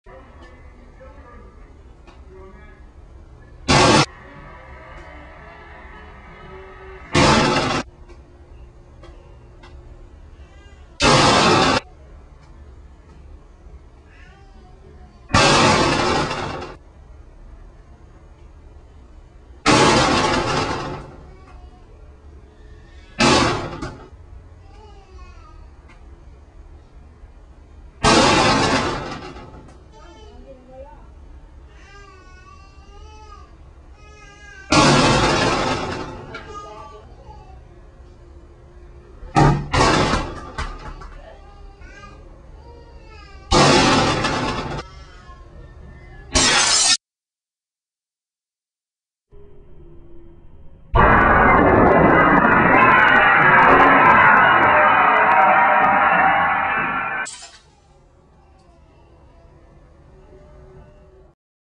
Attempting to Break a Window
I am trying to break a screen door window  by throwing a football through it.